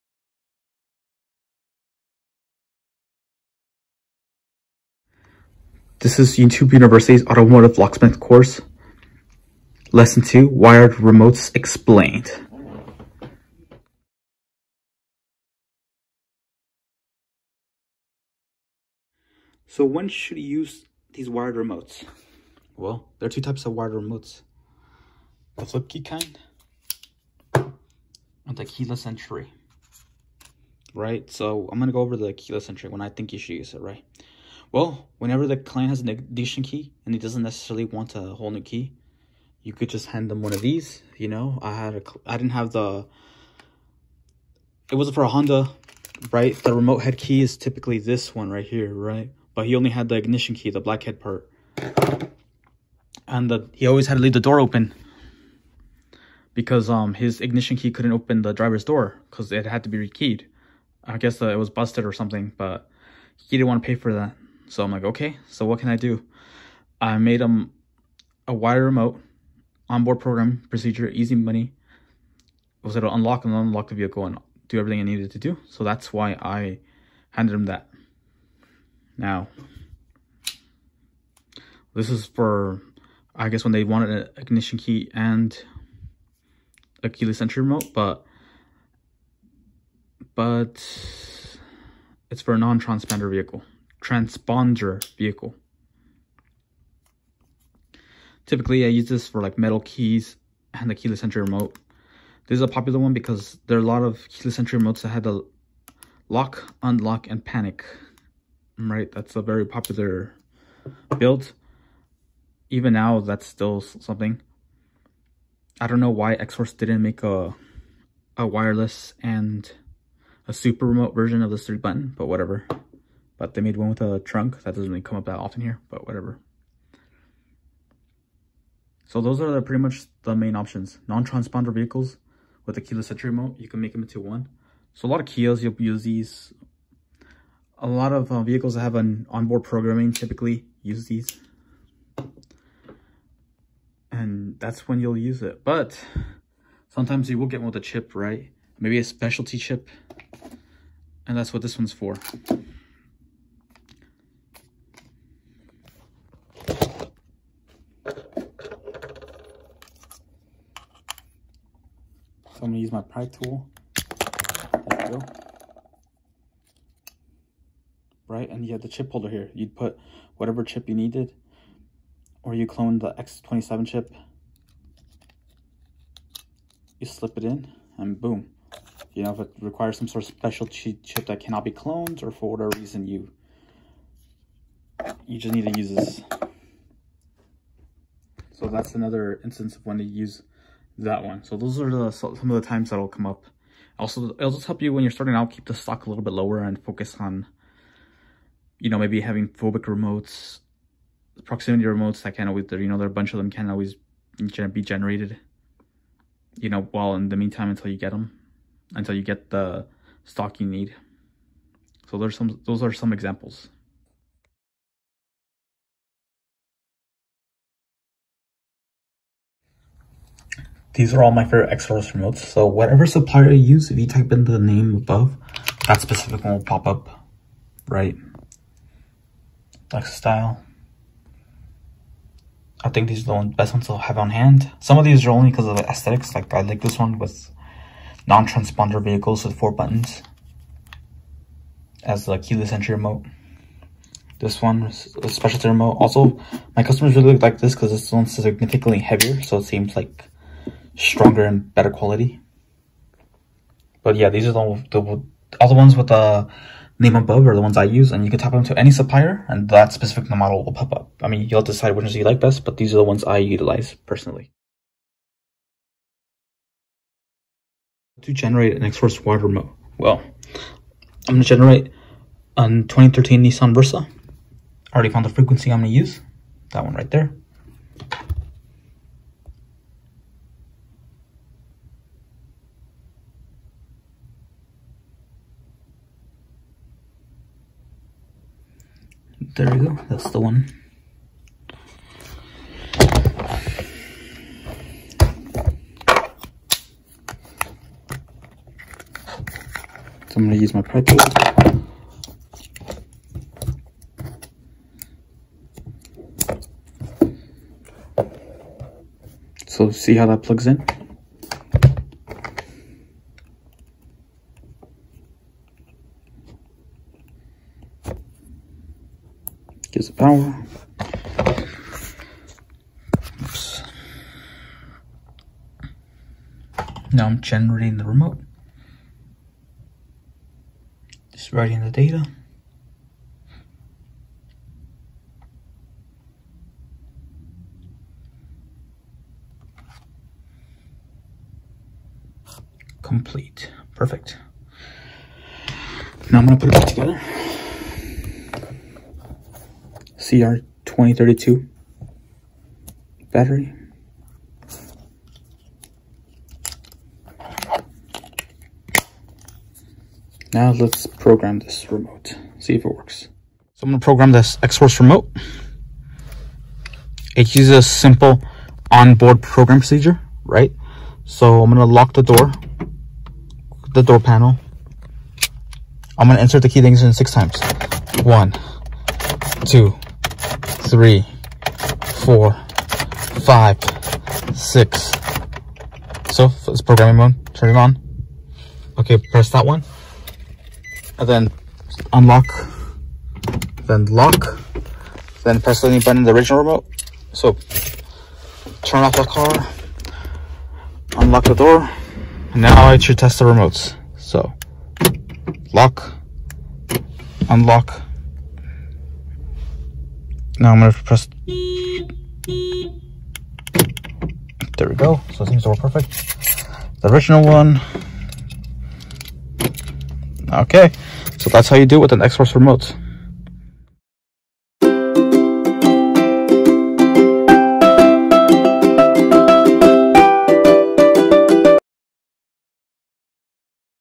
YouTube University Automotive Locksmith Course Universal key systems-Xhorse Wired Remotes Explained
00:00 intro
00:14 when to use wired remotes?
03:37 what are my favorite xhorse wired remotes
04:25 how to generate xhorse wired remote
07:03 outro

If you find my YouTube channel helpful, consider supporting the channel through one of these methods.